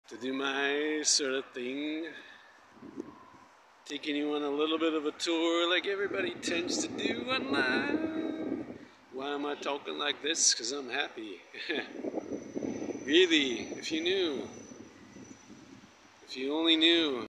Enjoying the day in nice weather - The weekend with Steve Hopkins
Enjoying the day in nice weather

w/Steve Hopkins